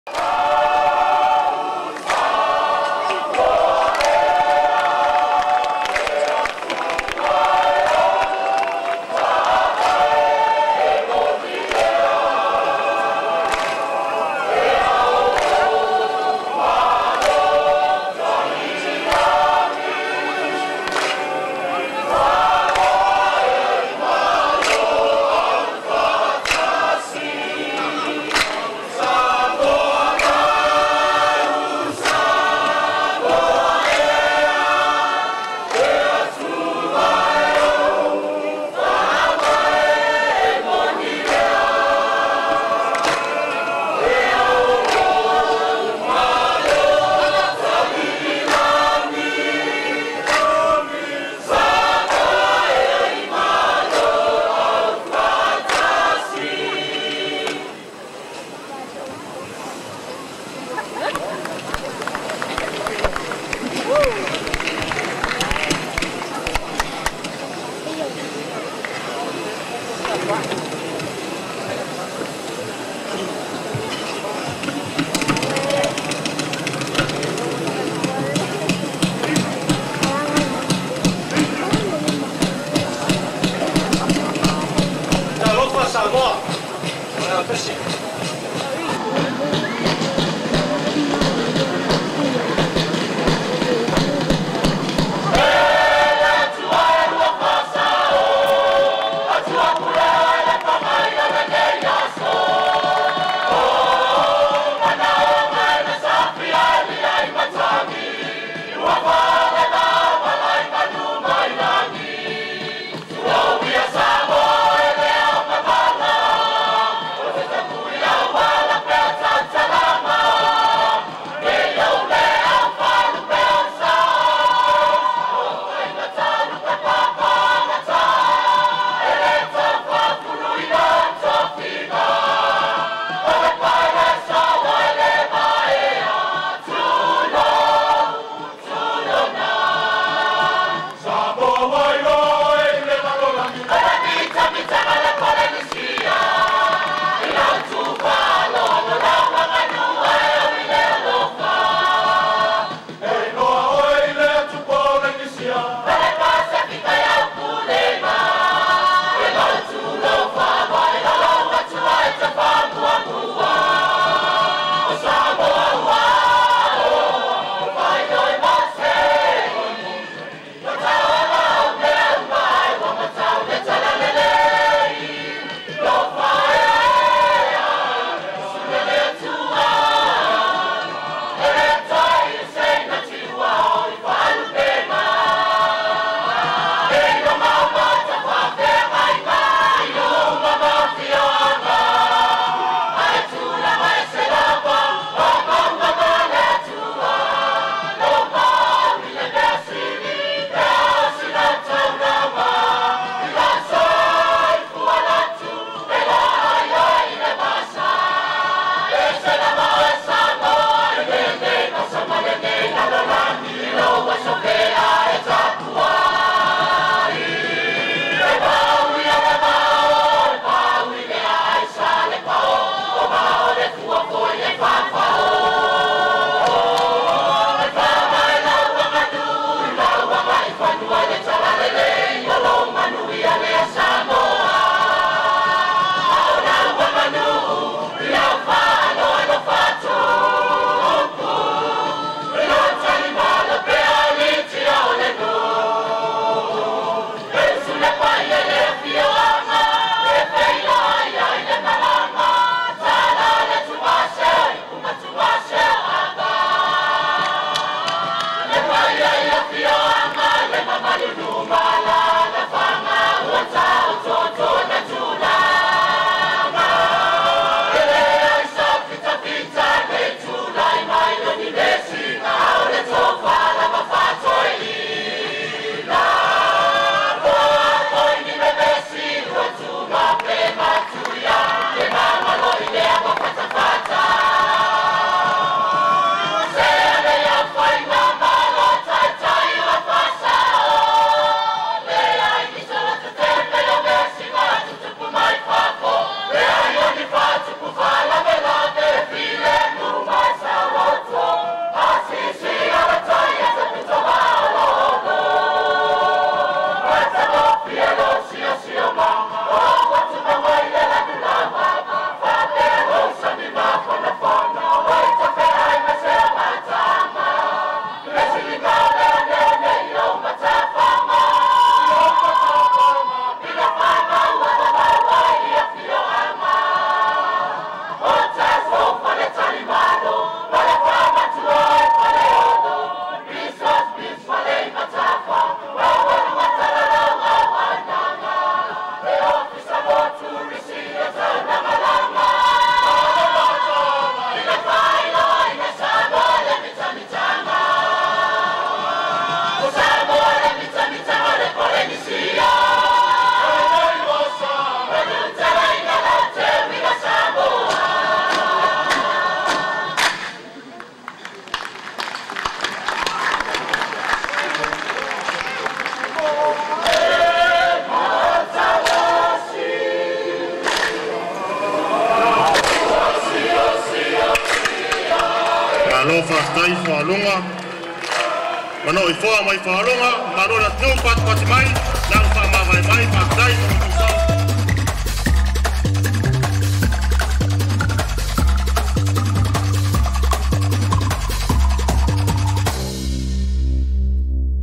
FOAILUGA (Mano'oifoa) : Pese fa'aleaganu'u
FOAILUGA (Mano'oifoa). Pese fa'aleaganu'u by FOAILUGA (Mano'oifoa) at the Teuila Festival 2005.
Produced by : Tuiātaga Fa'afili A.L Fa'afili t/a Video Aau